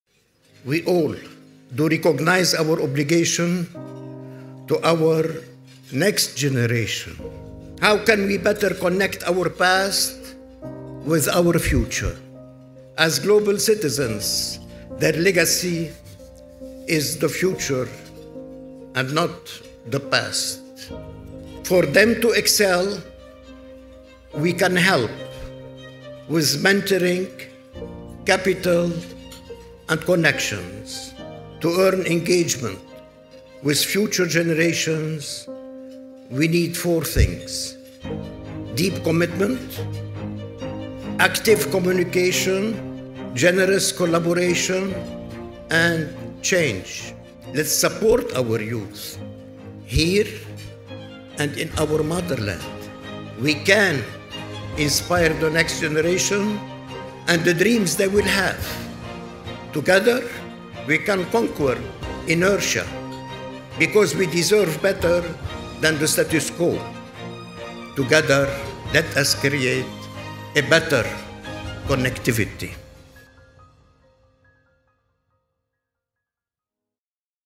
... Together let's create a better connectivity
Dr. Sfeir at the Annual Gala Dinner at ALCCI's 2018- Melbourne Australia